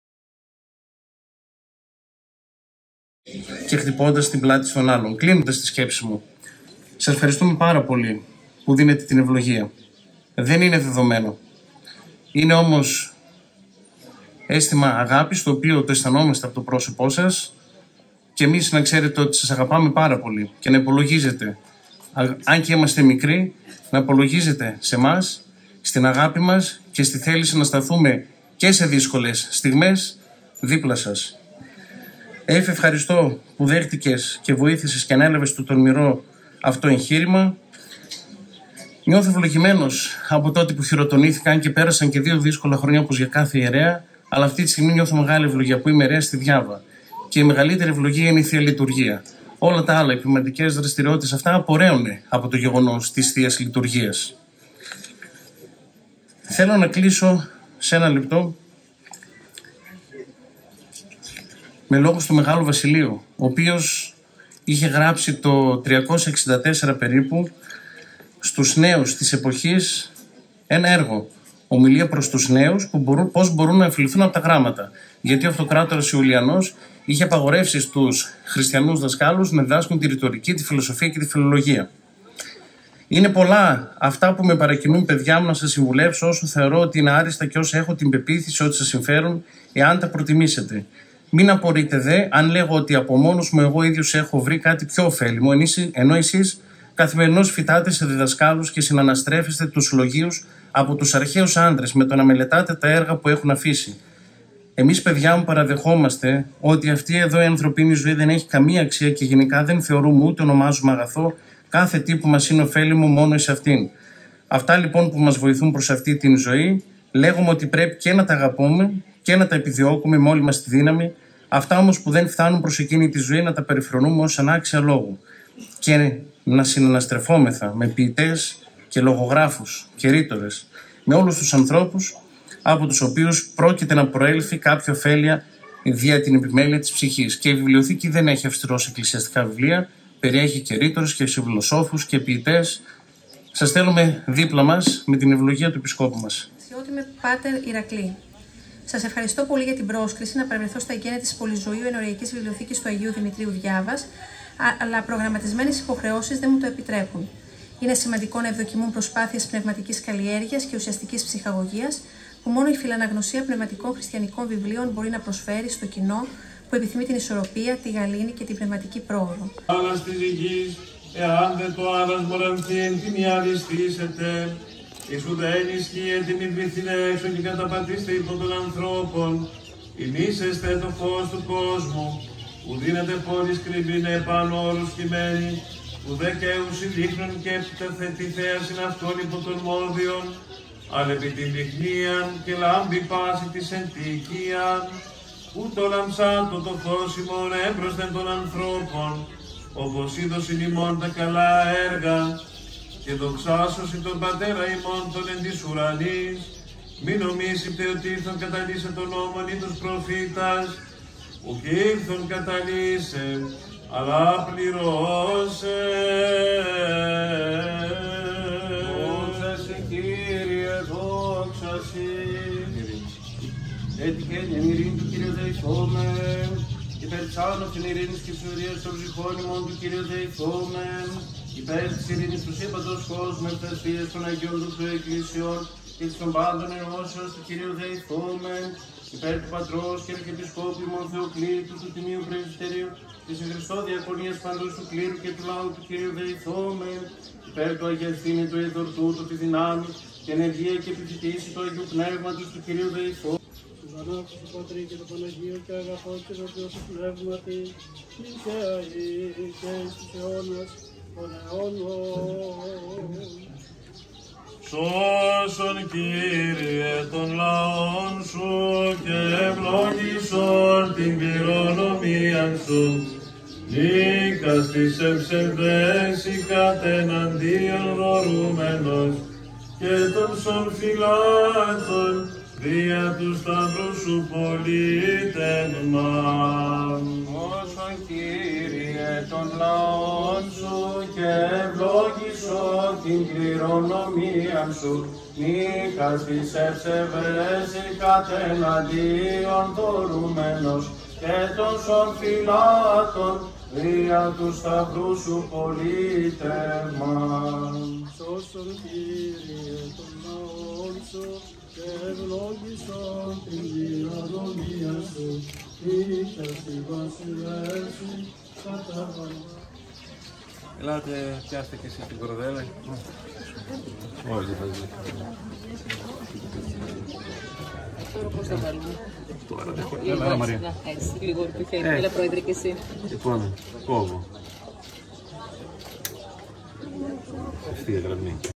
Εγκαίνια Ενοριακής Bιβλιοθήκης “Πολυζώειος” Αγίου Δημητρίου Διάβας
Καλαμπάκα: Τελέστηκαν τα εγκαίνια της ενοριακής βιβλιοθήκης “Πολυζώειος” του Αγίου Δημητρίου Διάβας
Σάββατο 27 Αυγούστου 2022

ΕΜΦΆΝΙΣΗ ΛΙΓΌΤΕΡΩΝ